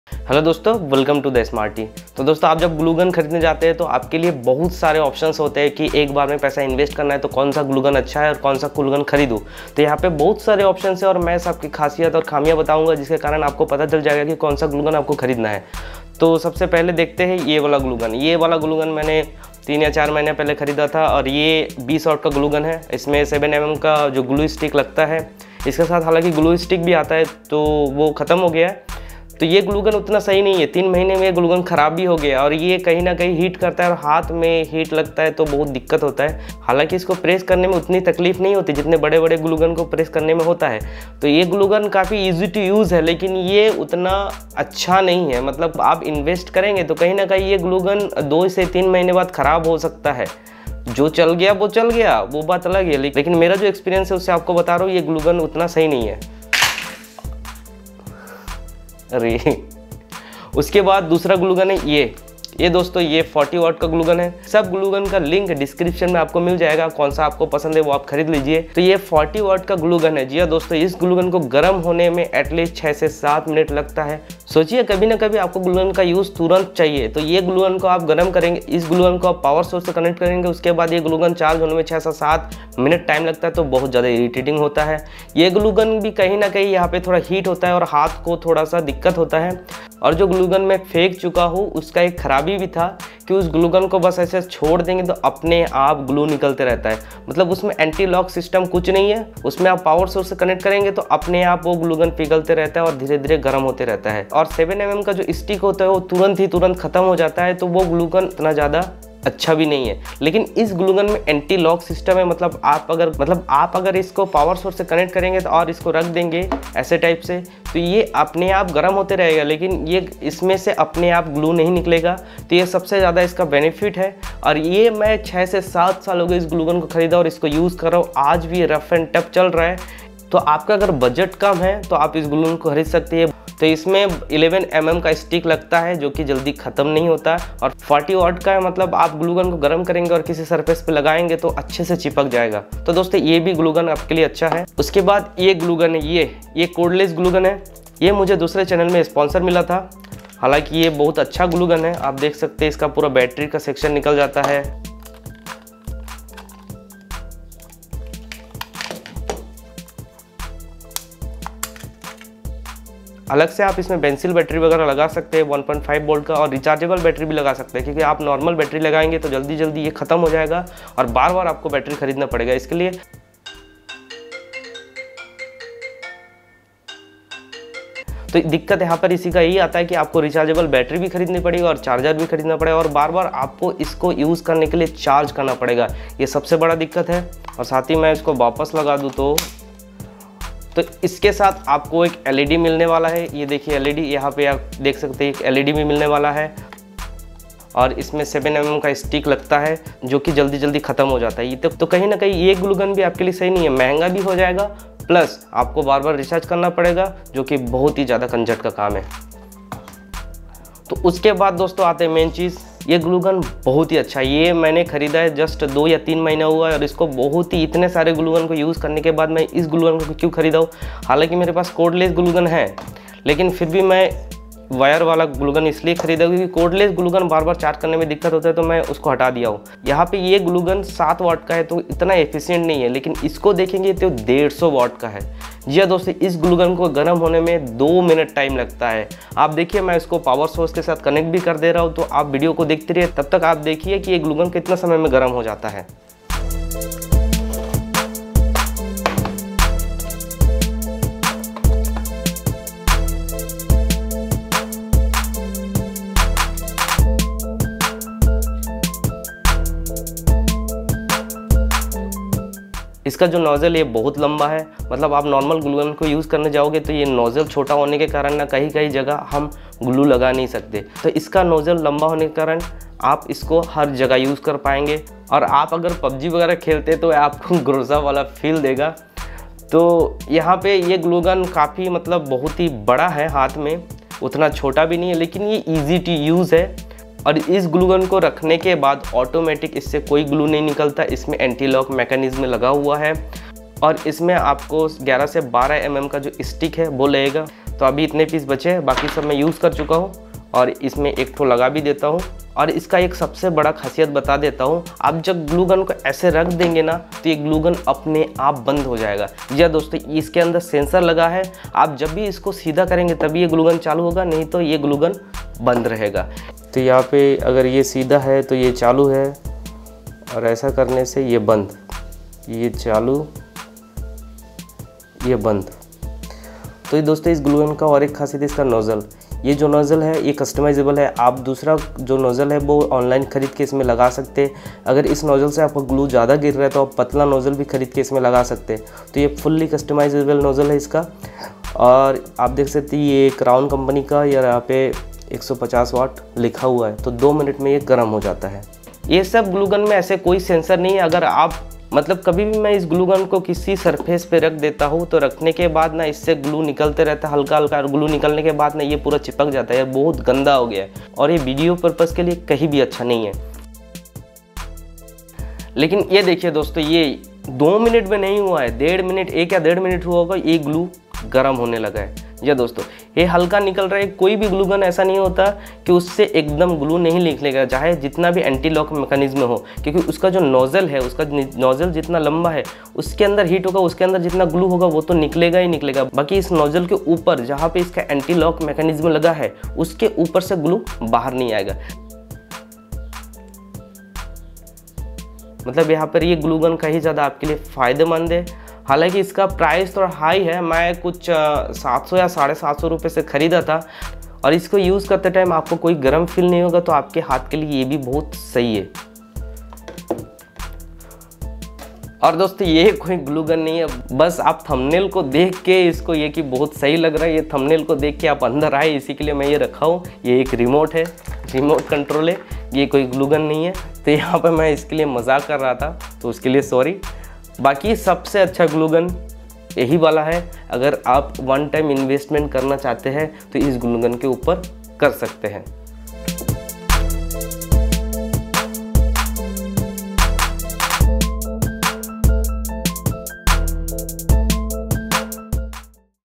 Which Glue Gun is Best for you? आपके लिए कौनसा ग्लू गन सही है? | The Smarty
Dosto main apko aaj glue gun ke bare me bataunga, aapko konsa gluegun kharidna chahiye

Mini Glue Gun
40W Glue Gun
Cordless Glue Gun
150W Glue Gun

Best glue gun for crafts
Best glue gun to buy
best glue guns 2020
best glue gun review